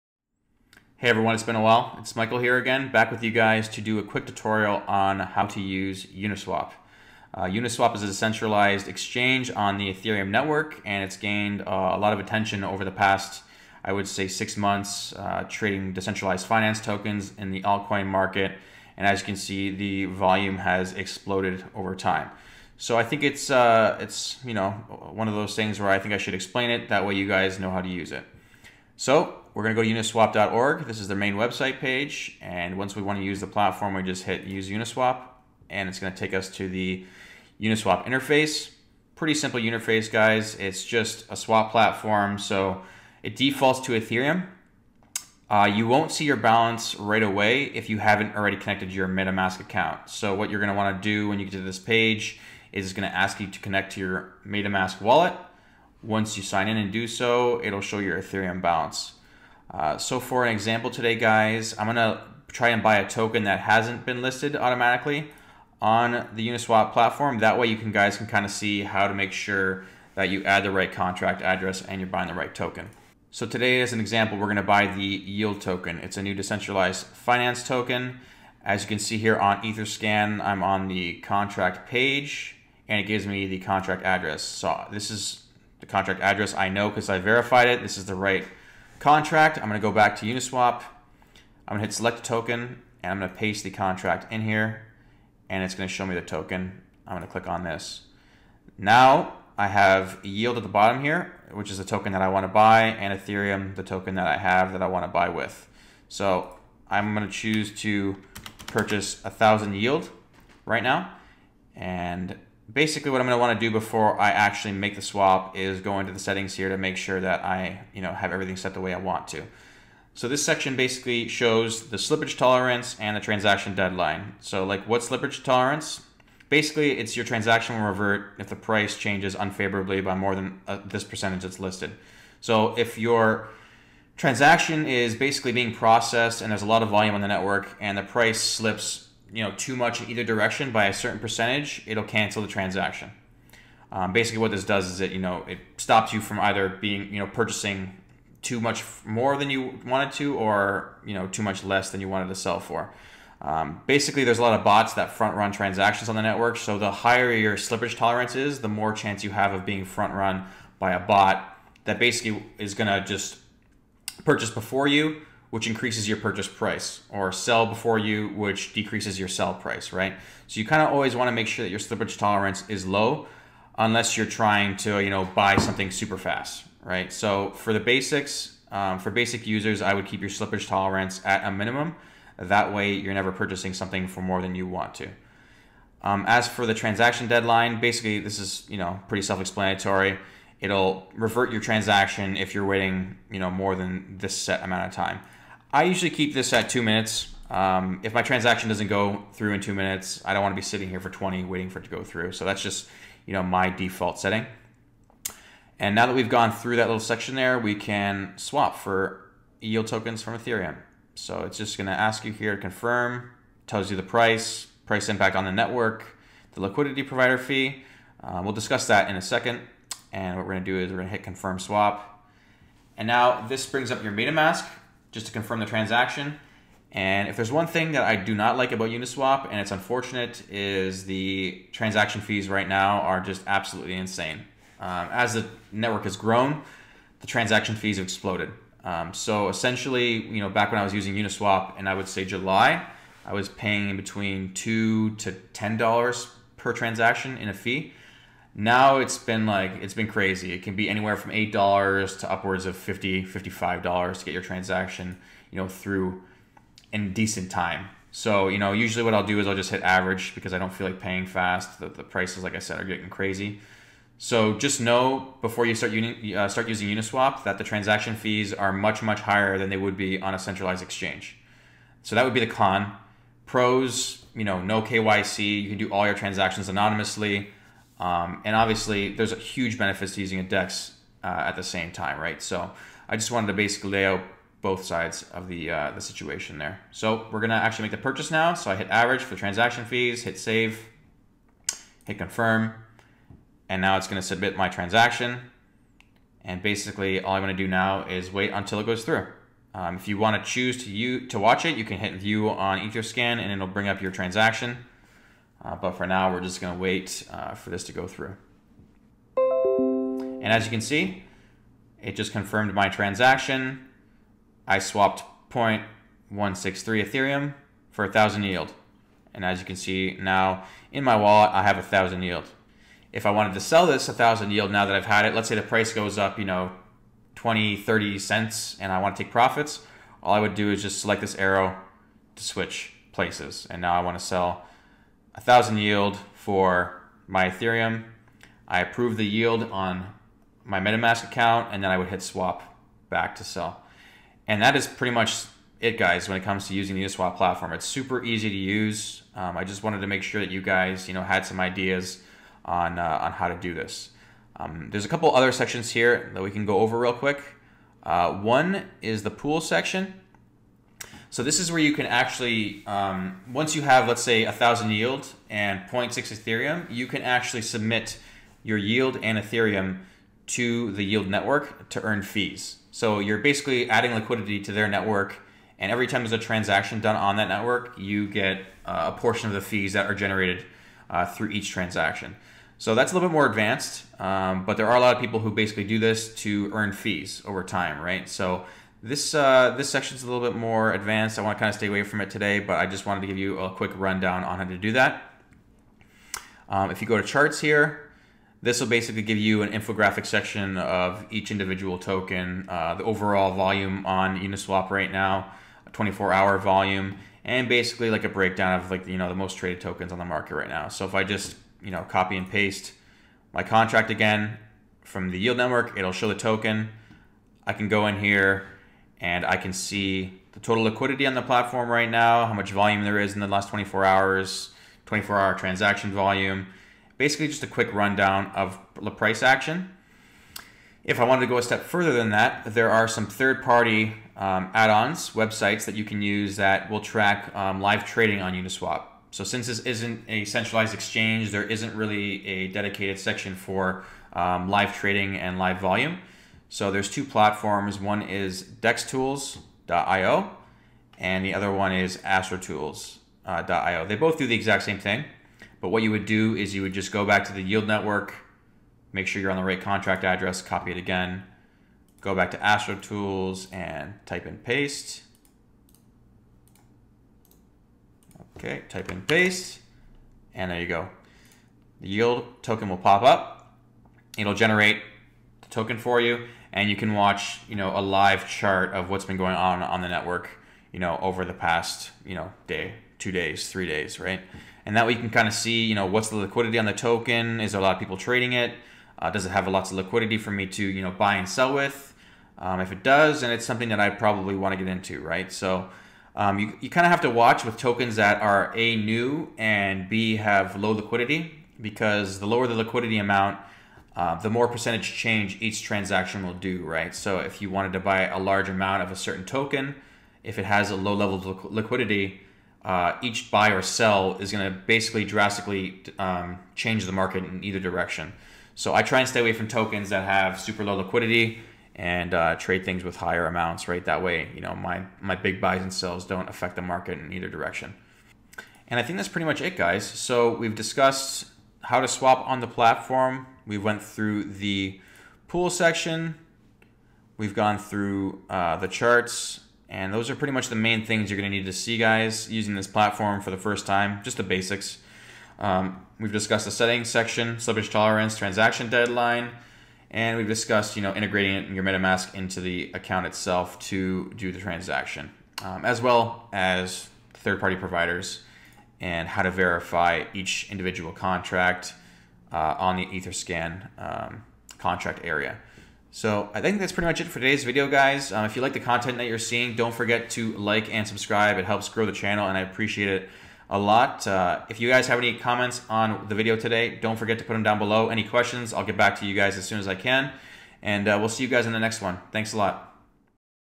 Uniswap Tutorial - How to Swap Tokens
In this video, I discuss the basics of how to swap tokens on the Ethereum Network using the Uniswap Decentralized Exchange and Metamask.

BTC: bc1qrep2ktn4xhxslmk8yuzxnmmdesz28czn7pfawn
ETH: 0x0a8AC85306eee8e804585a6042deFc19621A1a3d
LTC: LfTQBfZsY7teCvSEyxCwzqsqyffdtnTrWN
DOGE: D7WaL2QJiDUeqbgkcZCmAezNRcBXHVGvhY